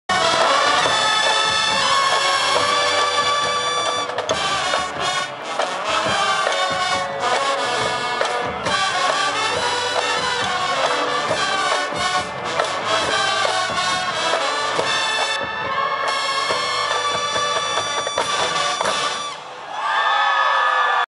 EWU Band FCS Championship
Eastern Washington's Band celebrates after victory in FCS Championship game vs Delaware at Frisco, TX January 7, 2011